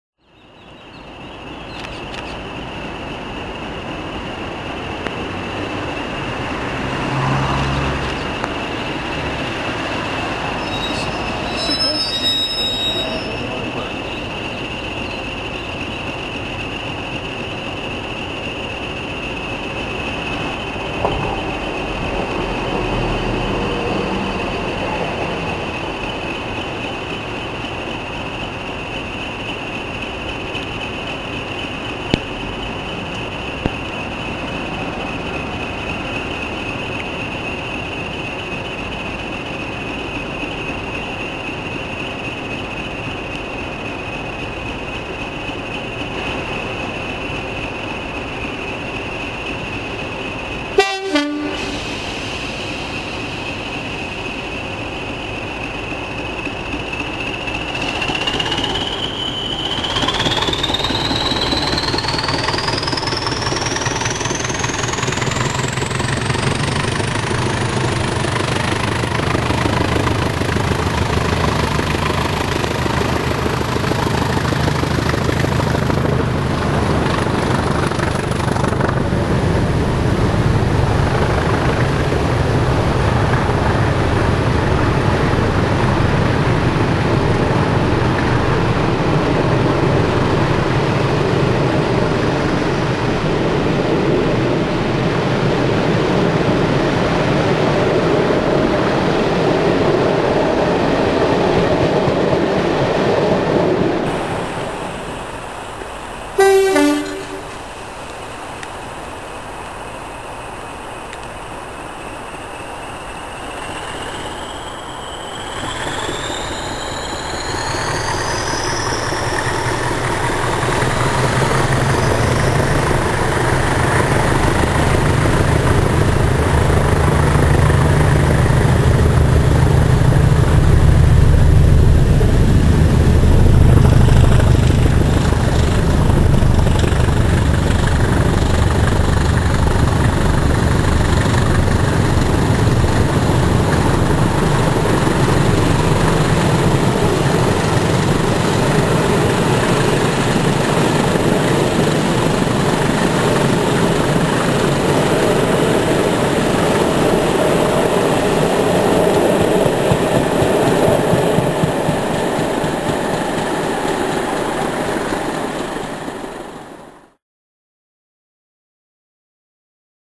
Snarling Class 20s depart Tame Bridge with the Torbay Flyer
1Z20 'The Torbay Flyer' arrives at Tame Bridge in this morning's rain around 10 minutes late, then blasts away with DRS choppers 20308+309 on full charge.

Friday 26th August 2011.